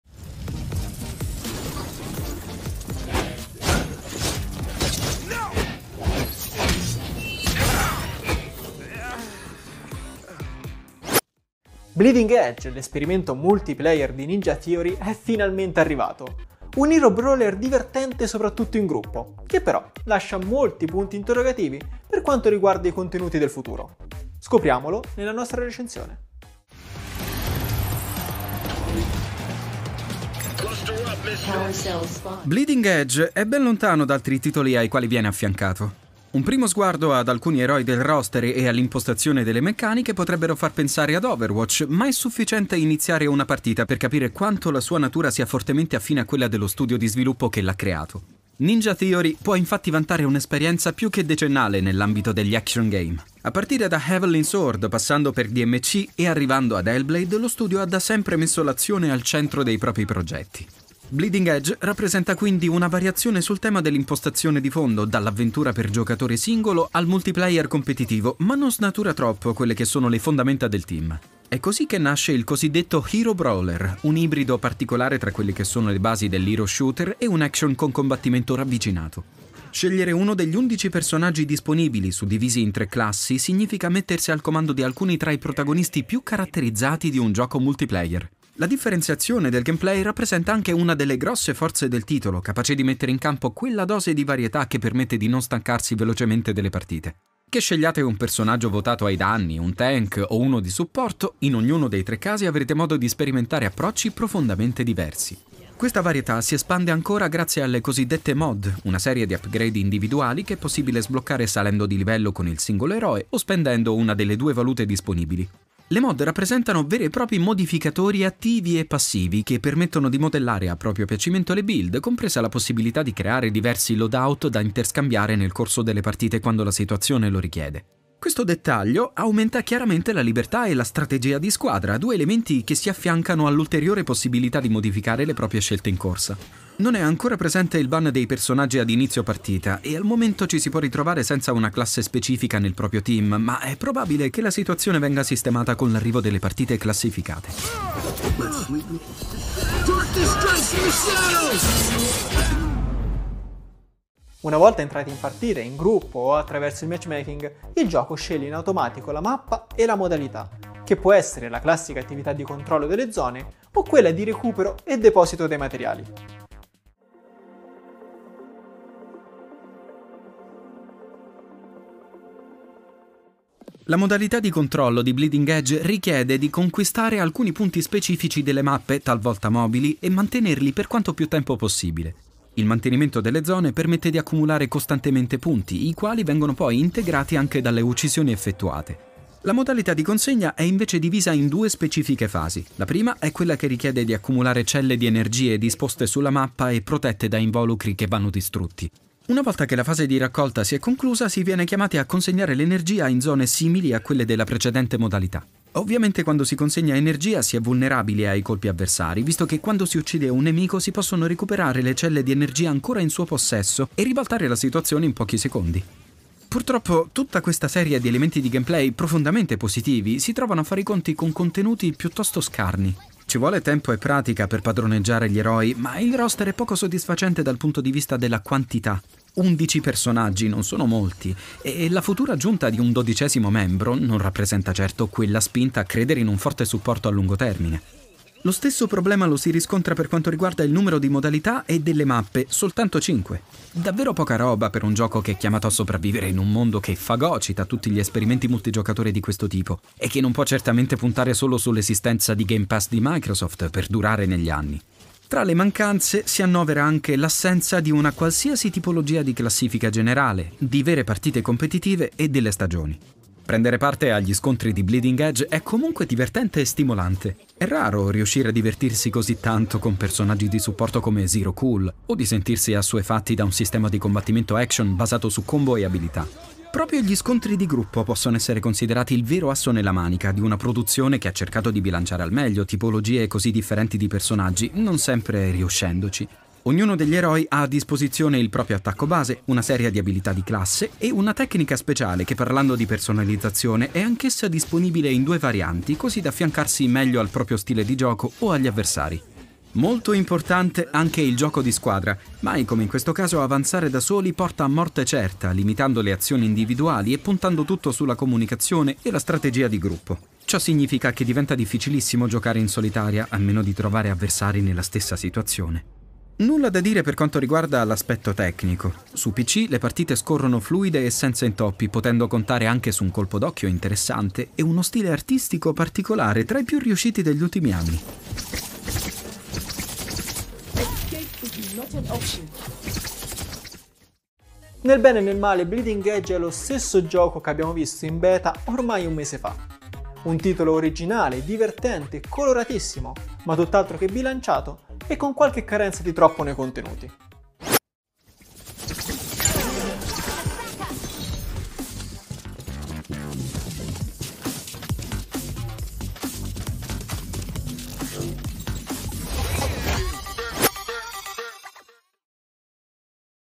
Bleeding Edge Recensione: originalità e incompiutezza!
Bleeding Edge, l'esperimento multiplayer di Ninja Theory, è finalmente arrivato. Un Hero Brawler divertente in gruppo ma con più di un punto interrogativo relativo ai contenuti e al futuro. Scopriamolo nella nostra recensione della nuova esclusiva Microsoft per Xbox One e PC.